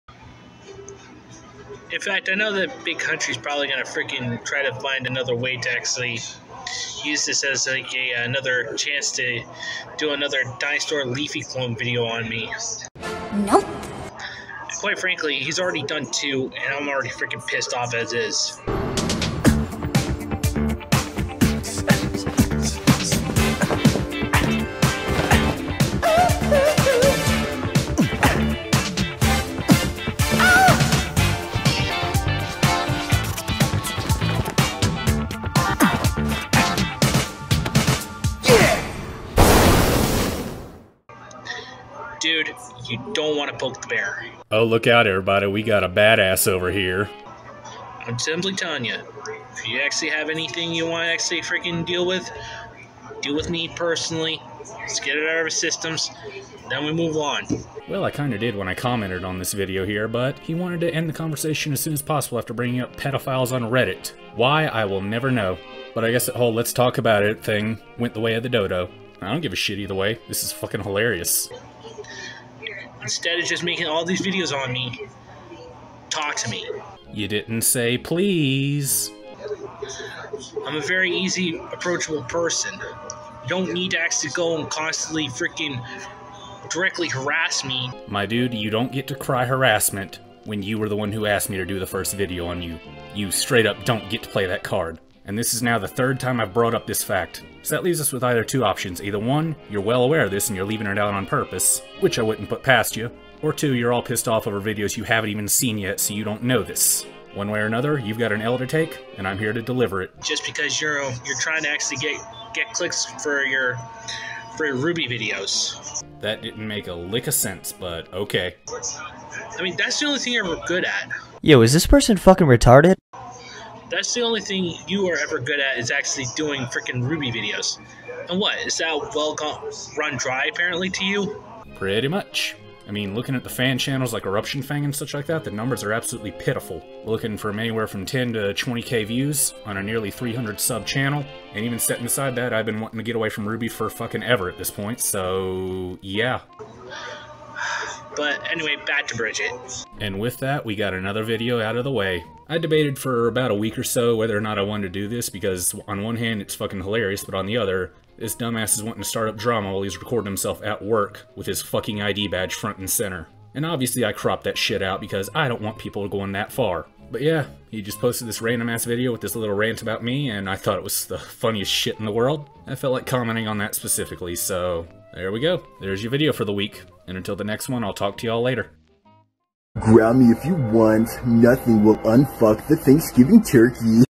I Have My Own Hippozoned Now, And He's Angwy :3
A channel I follow seems to have had enough of my shit, and is raging about me while he's at work ranting about different topics entirely. This does nothing but satisfy my sadistic needs tho :)